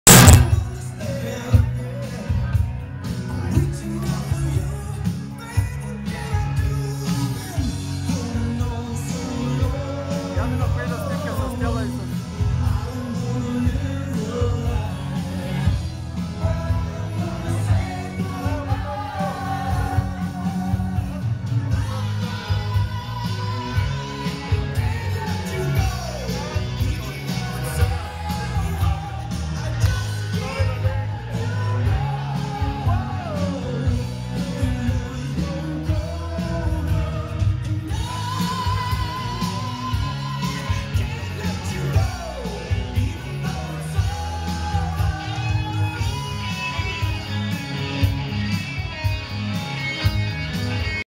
Dens dens dens......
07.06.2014 Среща на випуск 1984, при ТМТ"Харалампи Стоянов" - Горна Оряхоавицa